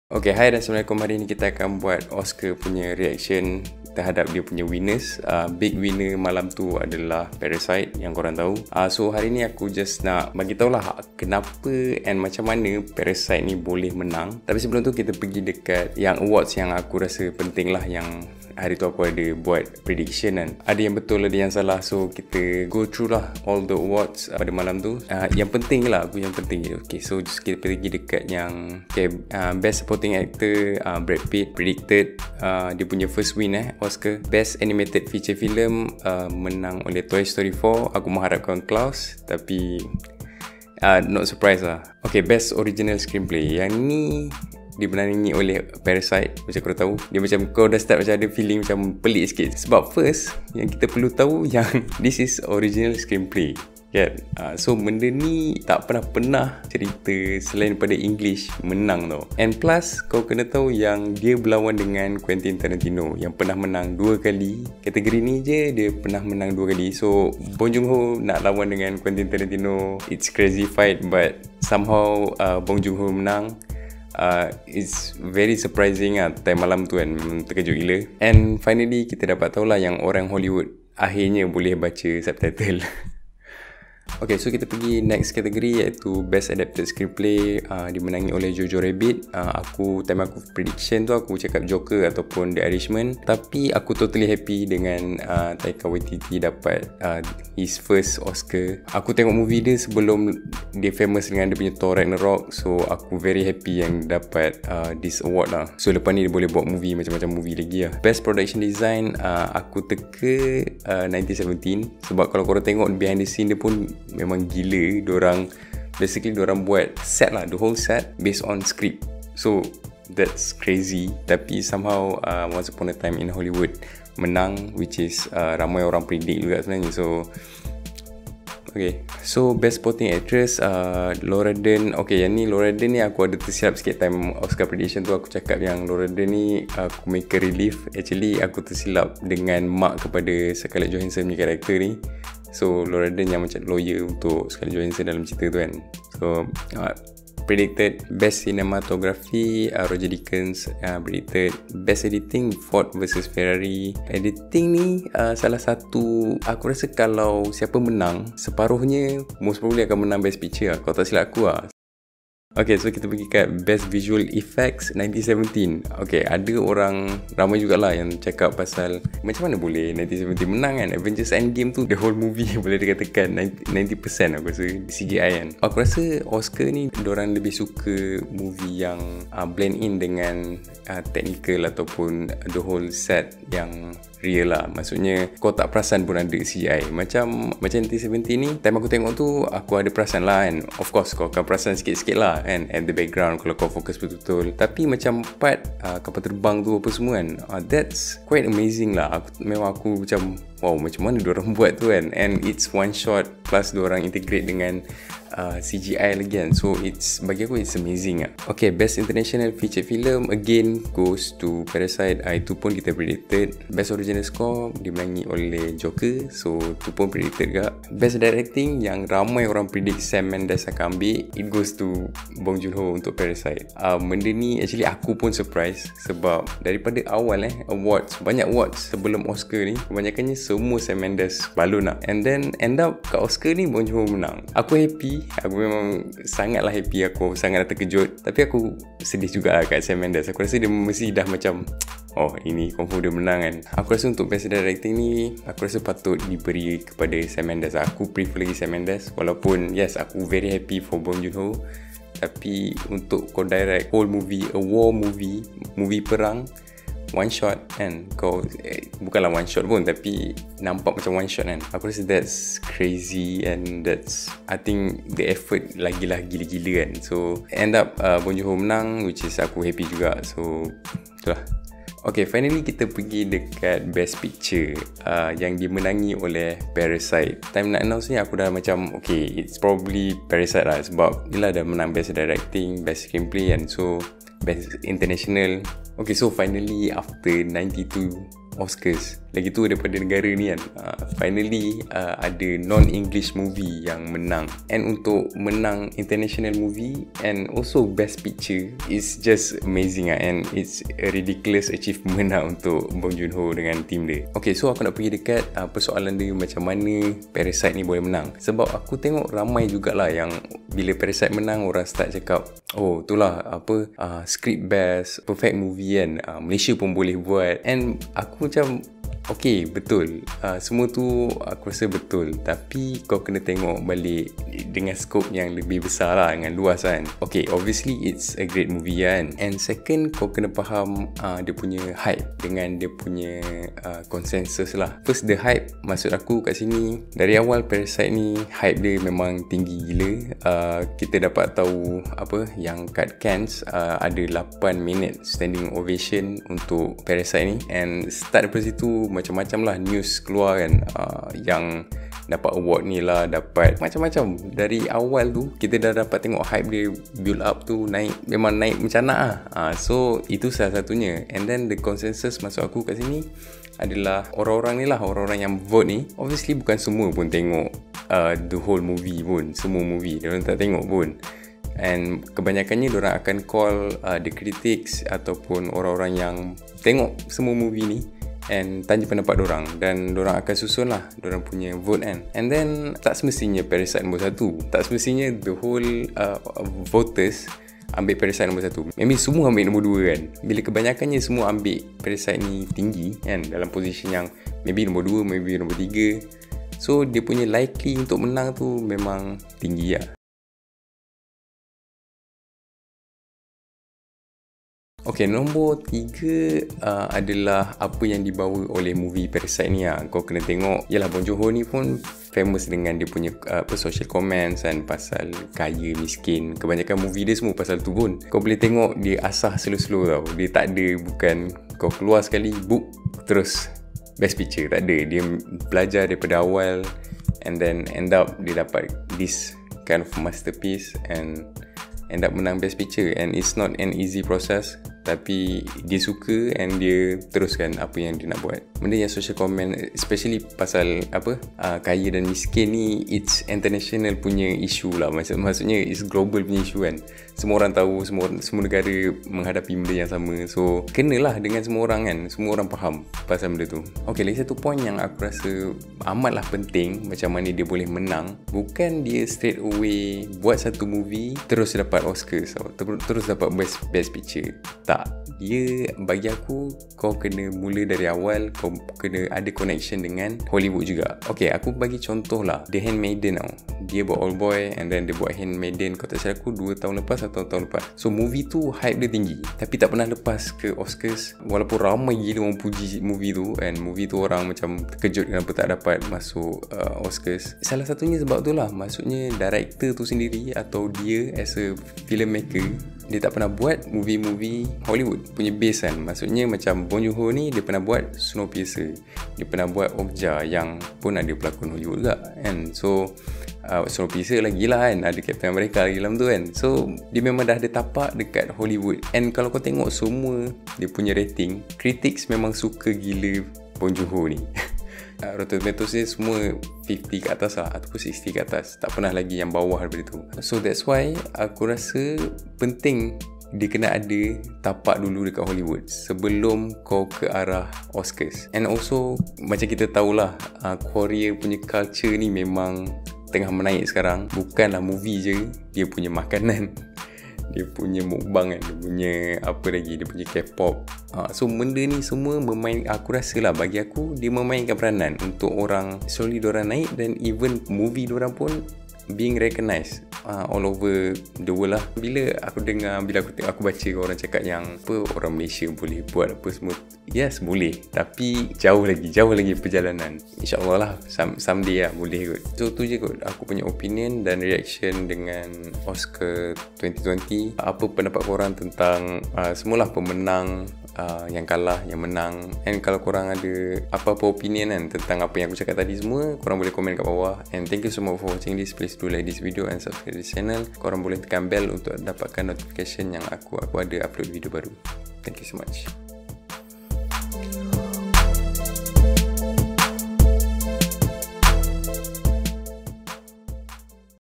Macam Mana 'Parasite' Boleh Menang? | Oscars 2020 | My Thoughts (HadamProject)
'Parasite' menjadi filem pertama Non-English movie yang dinobatkan sebagai pemenang besar Oscars sepanjang 92 kali dijalankan. Macam mana filem ni boleh menang? This is my thoughts and opinion on that matter.

Equipment:
1. Canon 200D/SL2
2. Fifine K670